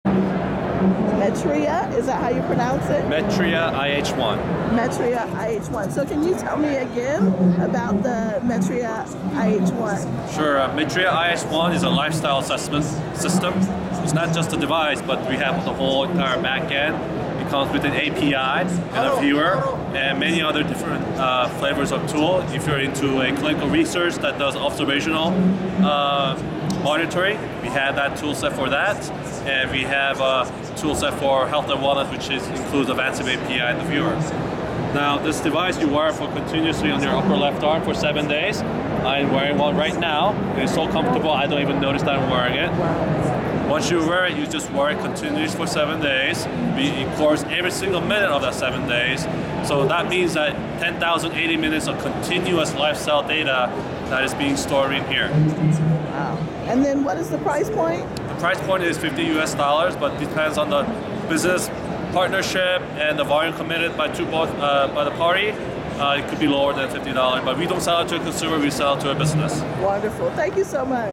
Metria IH1™ Digital Health Innovation from CES2015
Wear 7 days. Metria IH1™ collects 10,080 minutes of data and stores it until extraction and analysis.